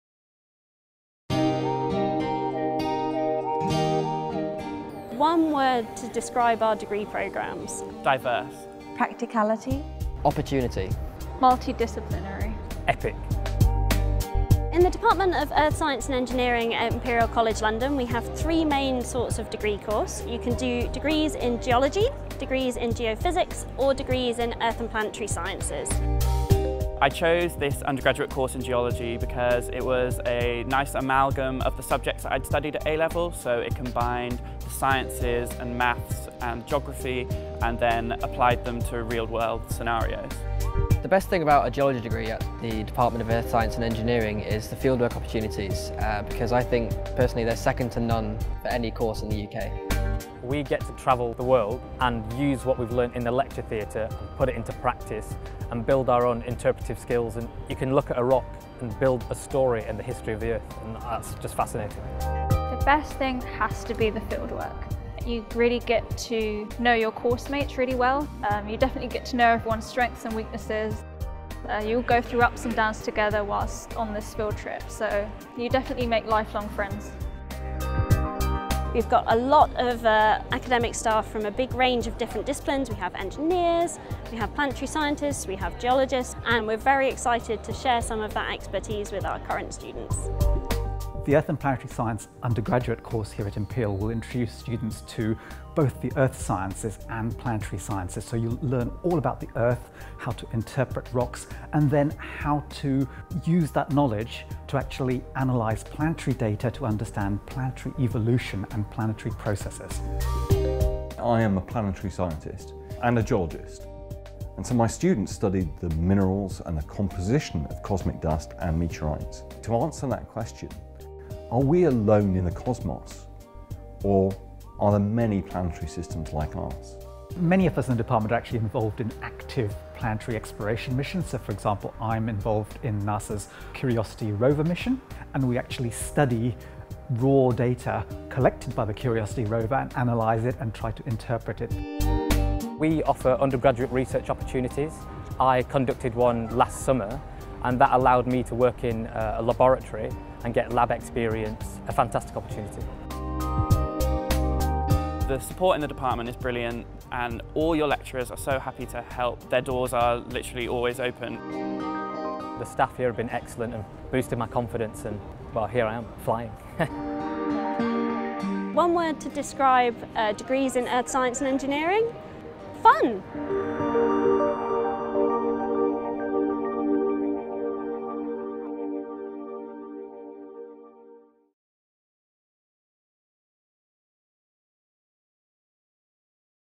Undergraduate Degrees, Department of Earth Science and Engineering
Hear from students and teachers about our Geology, Geophysics and Earth and Planetary Science programmes, including fieldwork and research opportunities